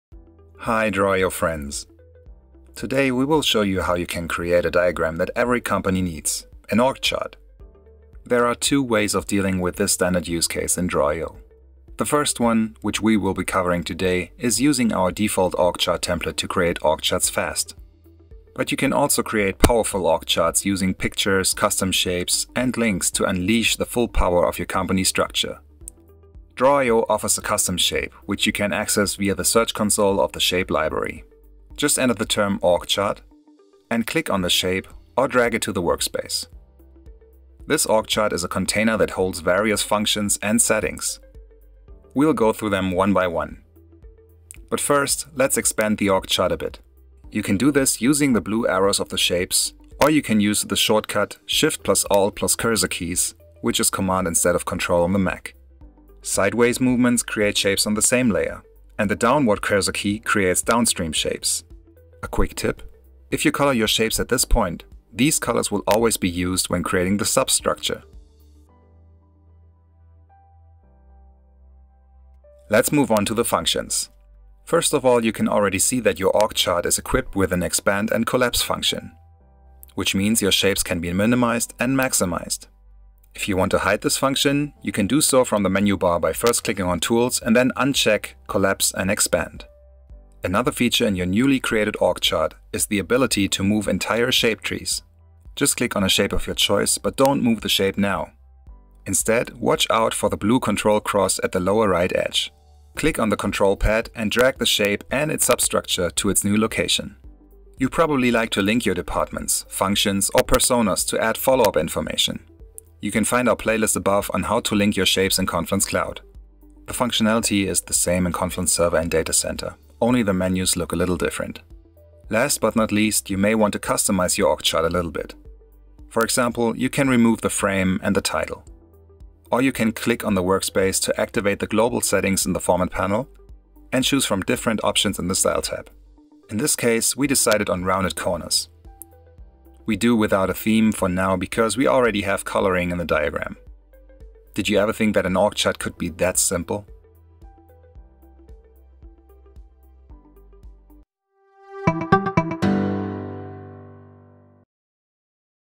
The easy way of creating org charts in draw.io
There are two ways of dealing with this standard use case in draw.io. The first one is using our default org chart template to create org charts fast. But you can also create powerful org charts using pictures, custom shapes, and links to unleash the full power of your company structure.